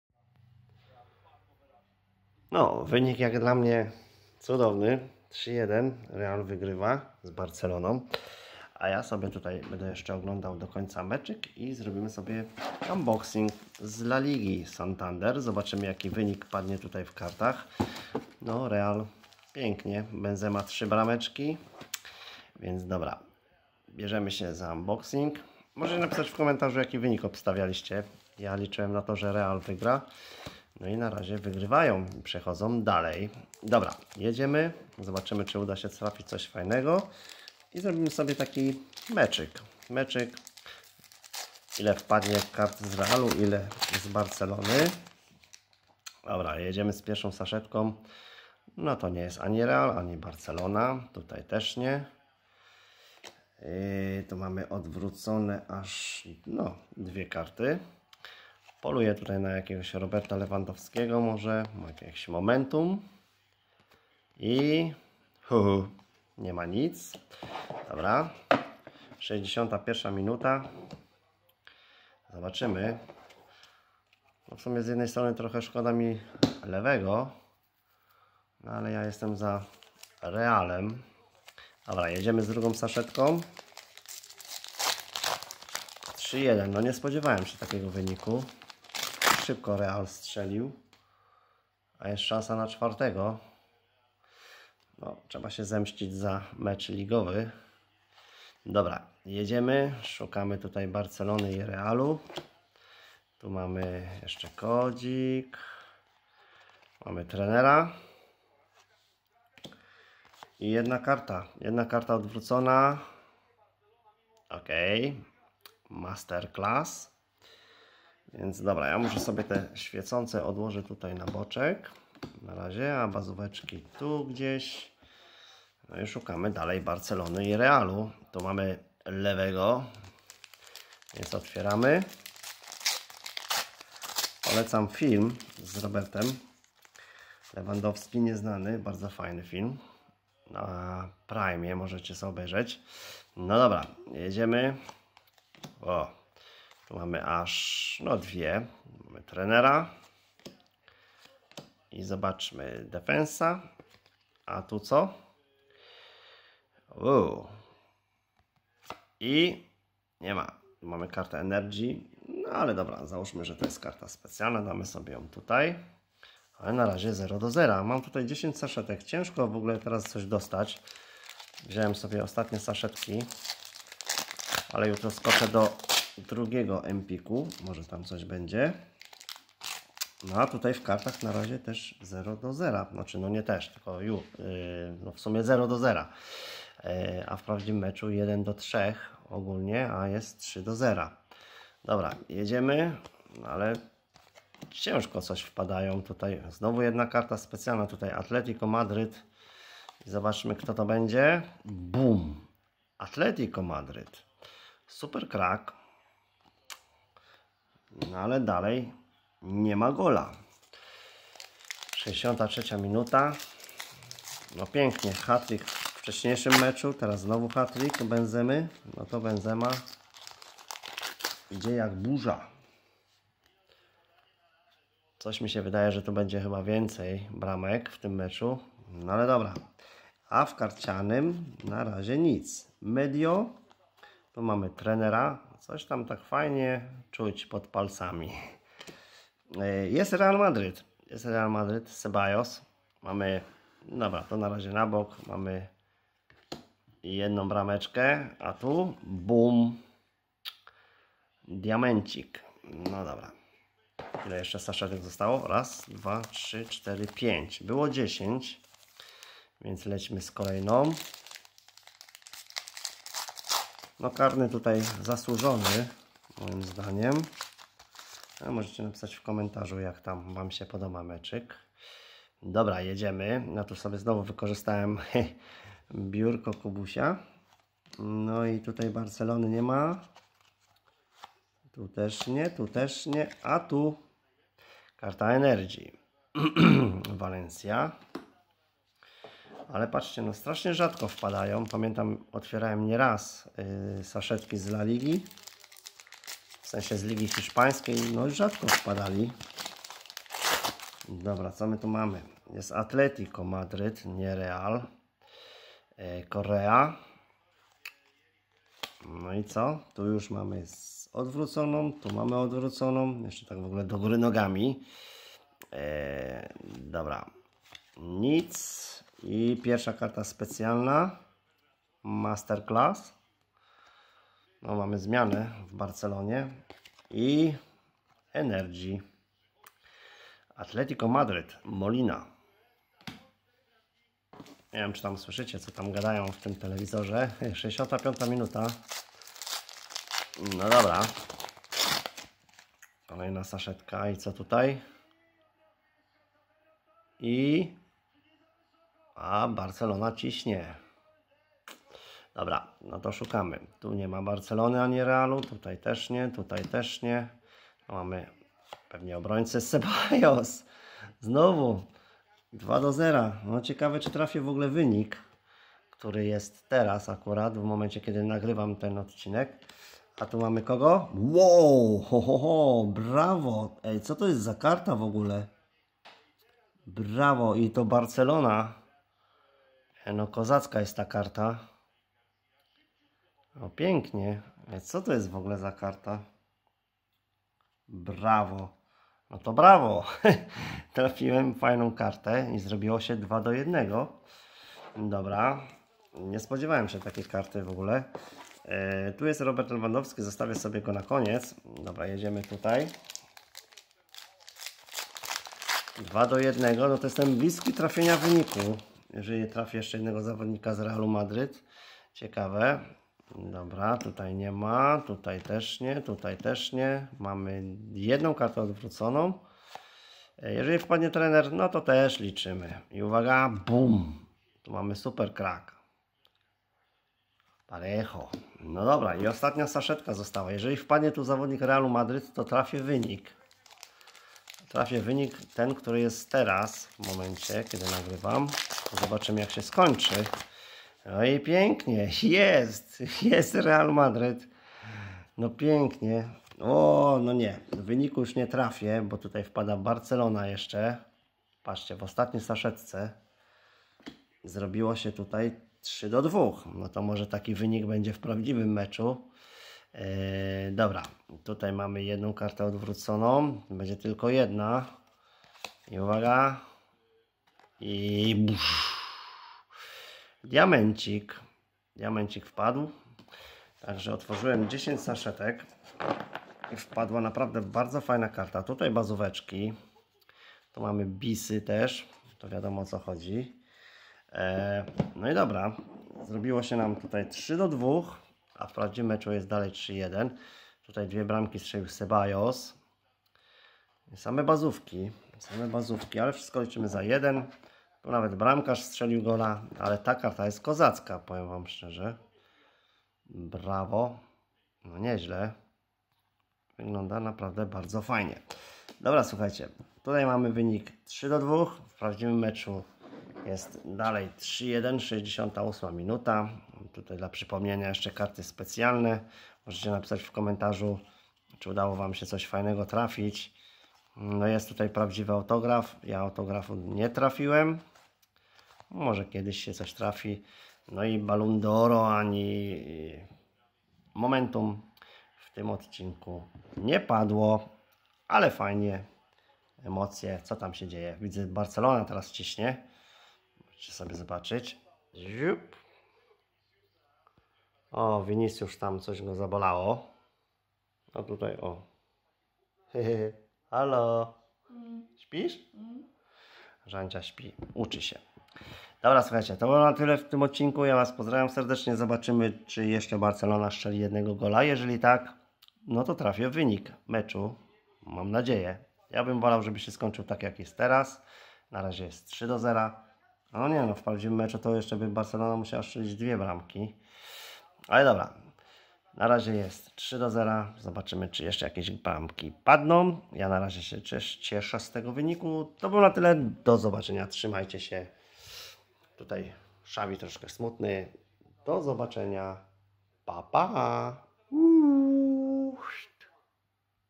HIT!!! UNBOXING PODCZAS OGLĄDANIA MECZU FC BARCELONA - REAL MADRYT (0-4) - WPADA MEGA KARTA
WOW!!! WPADA MEGA KARTA!!!